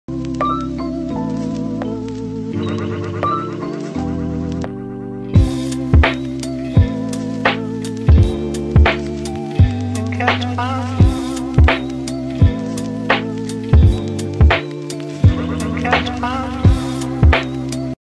Motion Page – Logo Animation
Happy to present the logo animation I created for Motion Page